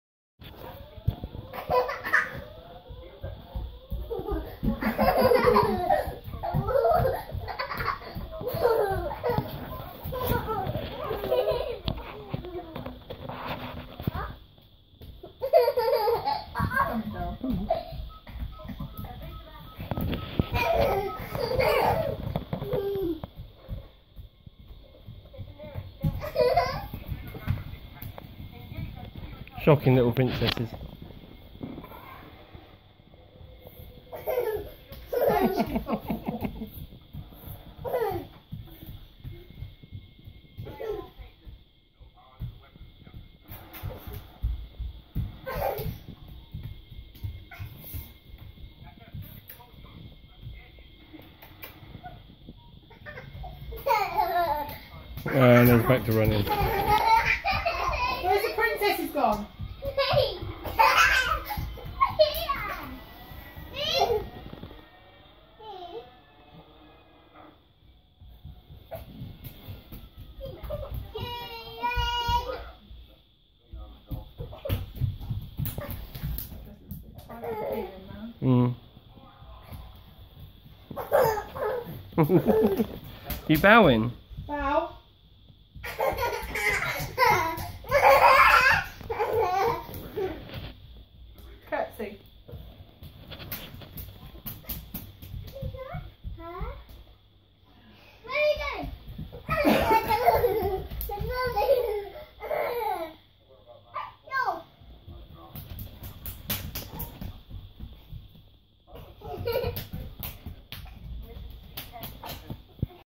Morrissey Girls 2015: Little Princesses
After their bath, and they were in their Sofia the First nighties, Erin & Keilyn started acting like proper little princesses.